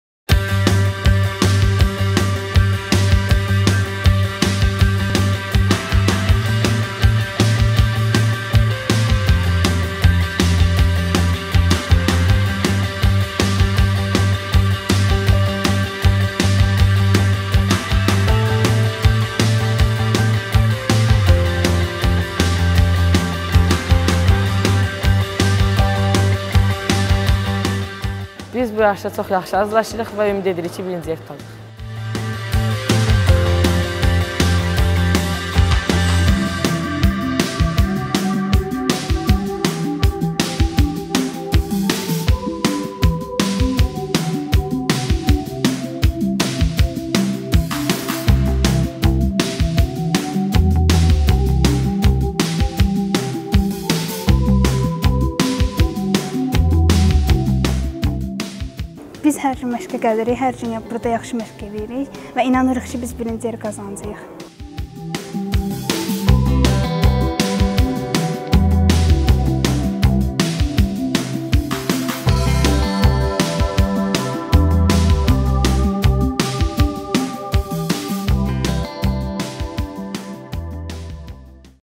Nəsimi rayonu - Həndbol
"OMA" layihəsi çərçivəsində Nəsimi rayonunda baş tutmuş məktəbdaxili həndbol yarışından həyacanlı anları sizinlə bölüşürük.